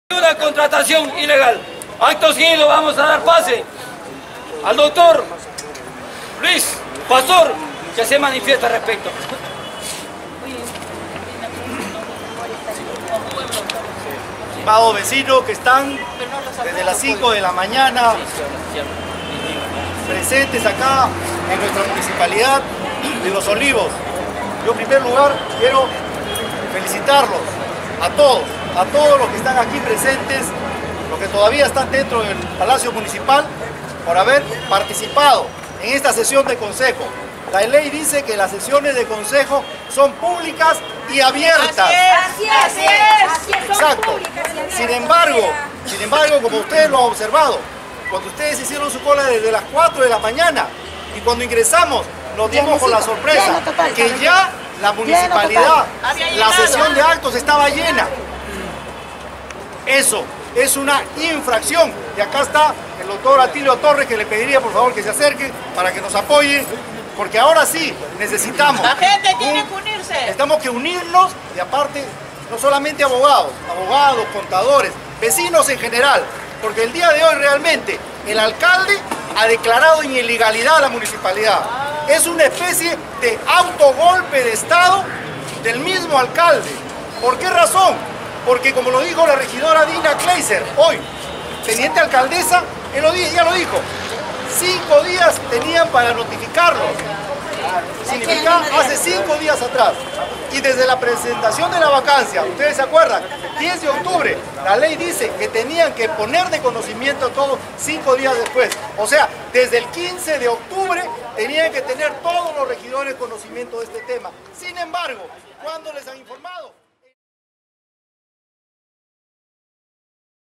FRACASO VACANCIA EN LOS OLIVOS
Con 9 votos en Contra de la vacancia, 3 a favor y una abstención, fue el resultado del pedido de vacancia contra el alcalde de los Olivos Pedro del Rosario, algunos regidores se pronunciaron en contra del pedido, porque carece de fundamentos y ademas tiene tinte político de ex candidatos perdedores, que quieren desgastar a la actual gestión, para favorecer sus futuras candidaturas a la alcaldía de los Olivos.